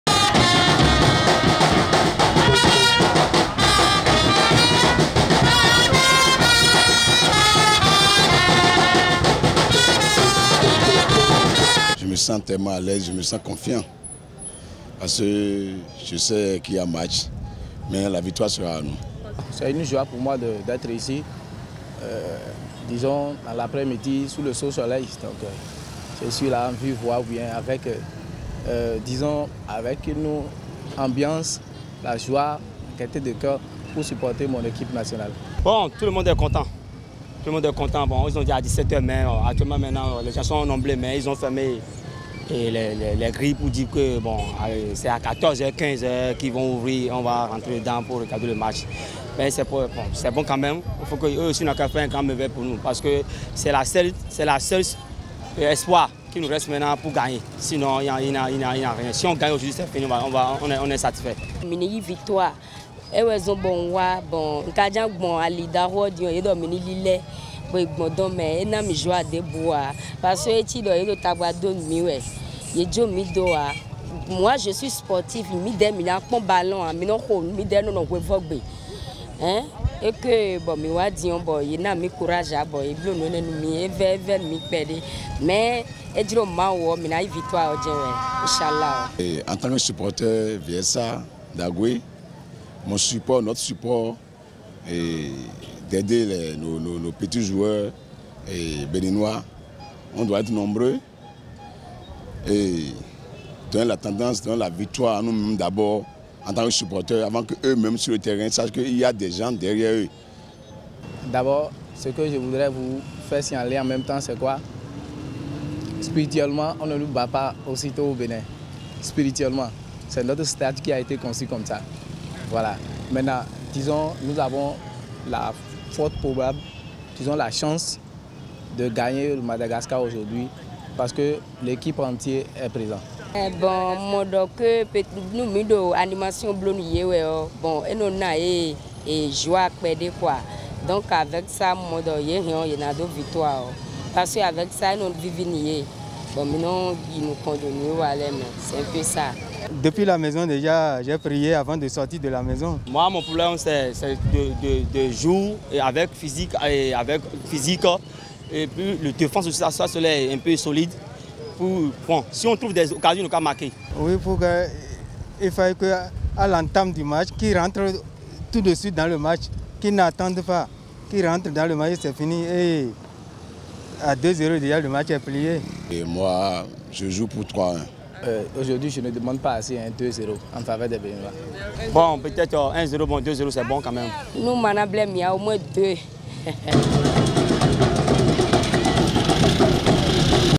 BÉNIN MADAGASCAR VIVEZ L'AMBIANCE AU STADE DE L'AMITIE GMK
Esae tv vous propose de vivre l'ambiance qui prévaut au Stade de l'amitié Général Mathieu Kérékou, à quelques heures de la rencontre footballistique opposant le Bénin au Madagascar. Un match qui s'inscrit dans le cadre de la 5ème journée des éliminatoires de la coupe du monde Qatar 2022.

Nous sommes également disponible sur les smart-TV partout au monde via l'application YouTube. Entrer ESAE pour accéder à nos émissions depuis votre poste téléviseu